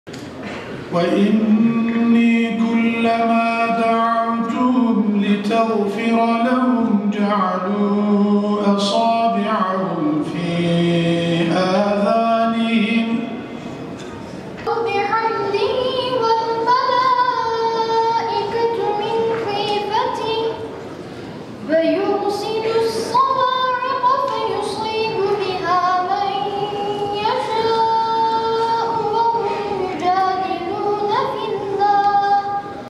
“Hafızlık Yarışması Türkiye Finali” Diyarbakır'da yapıldı
Diyanet İşleri Başkanlığı tarafından tertiplenen “Hafızlık Yarışması Erkekler Türkiye Finali” Diyarbakır'da gerçekleştirildi. Kur'an-ı Kerim'i ezberleyerek yarışmaya katılmanın heyecan ve mutluluğunu yaşayan hafızlar, güzel sesleriyle okudukları haşır ile katılımcıları cezbettiler.